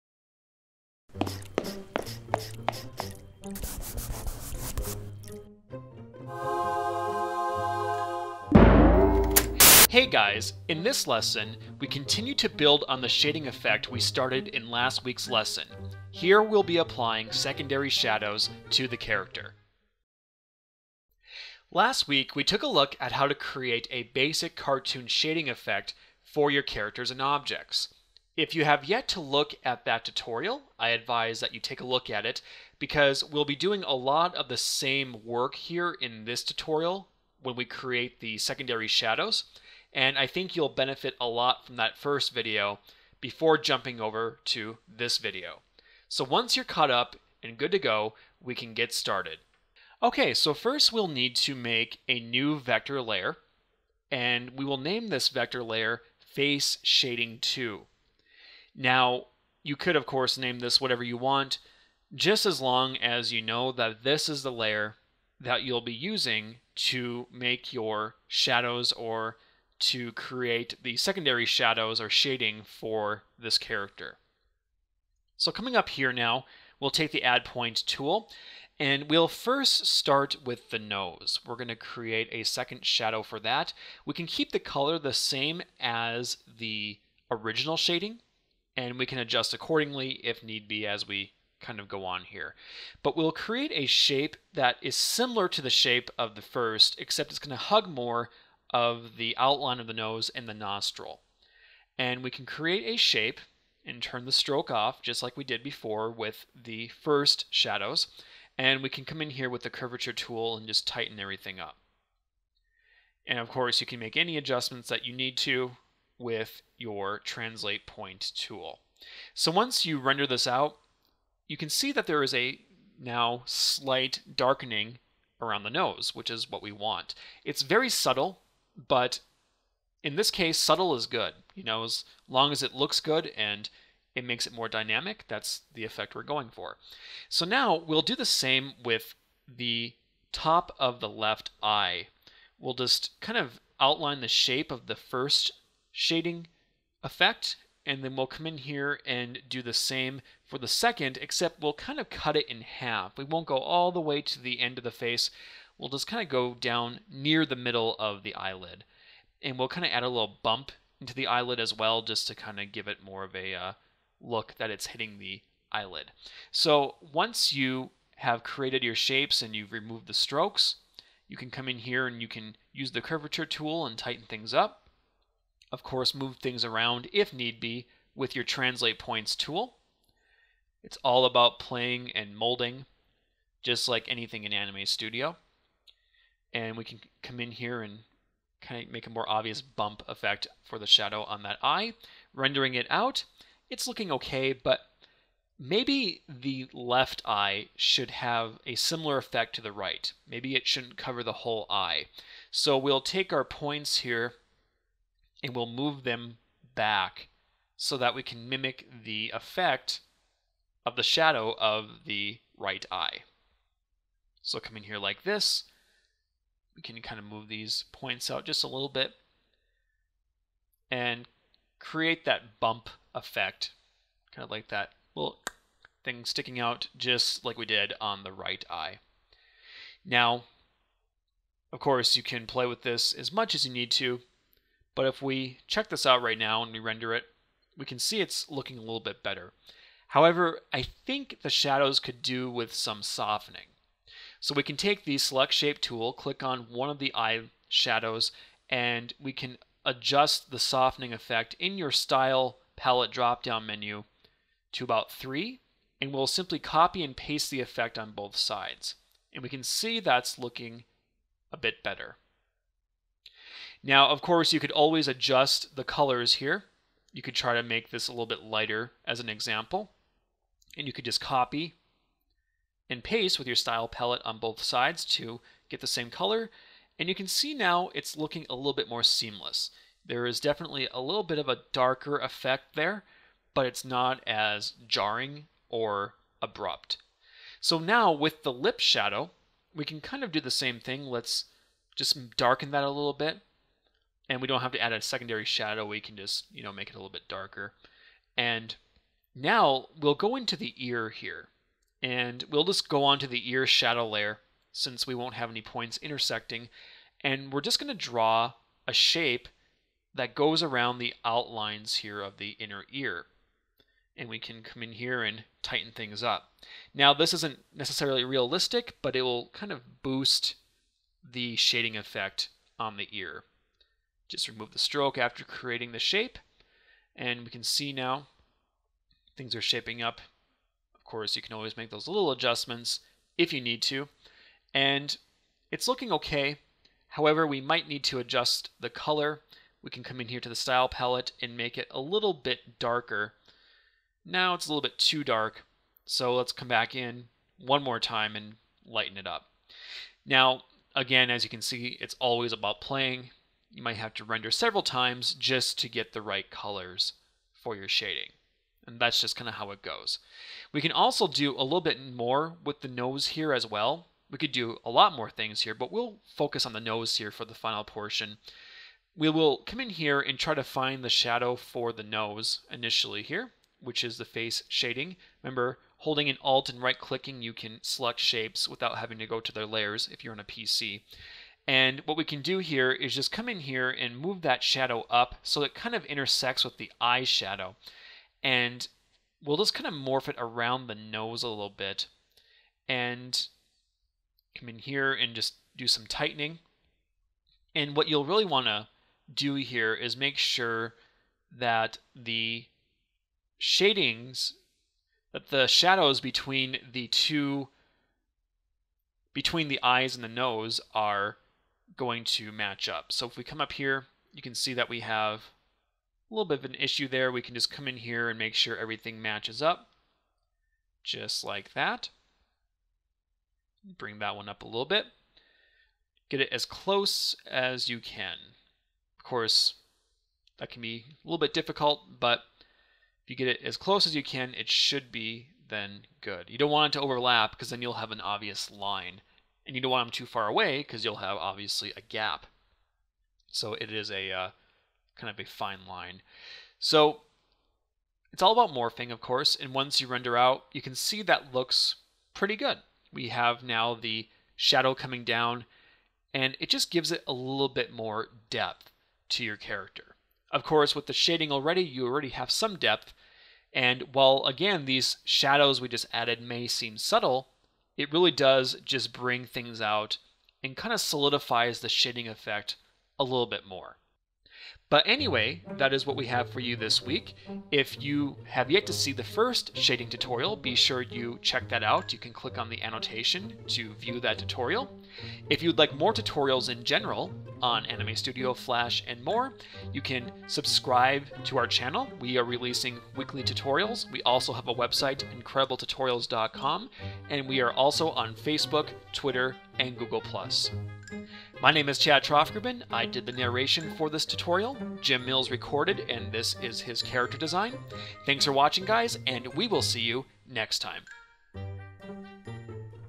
Polishing Up Facial Shading in Anime Studio
Need a copy of Anime Studio?

In the second part of our series on shading, we examine how to apply secondary shadows to our character.

Software Required: Anime Studio Pro
Skill Level: Beginner
Recorded By: Jim Mills
Narrated By: Chad Troftgruben